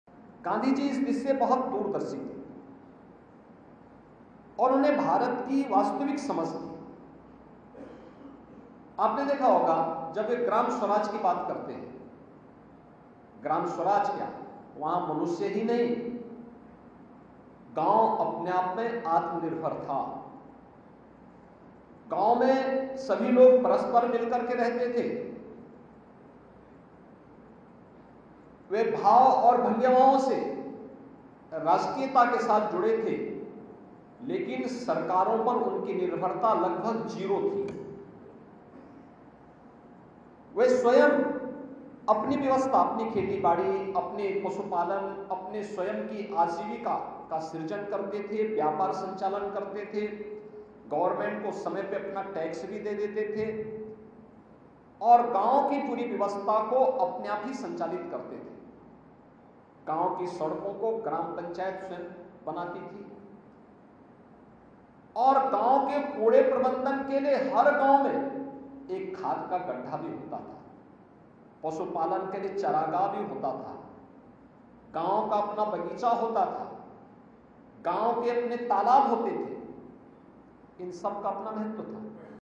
गांधी जी इस दृष्टि से बहुत दूरदर्शी थे...
आज गोरखपुर में दिग्विजयनाथ स्नातकोत्तर महाविद्यालय एवं भारतीय सामाजिक विज्ञान अनुसंधान परिषद (ICSSR), नई दिल्ली के संयुक्त तत्वावधान में आयोजित 'पर्यावरण, प्रौद्योगिकी एवं सतत ग्रामीण विकास' विषयक दो दिवसीय राष्ट्रीय संगोष्ठी के उद्घाटन समारोह में सहभाग किया।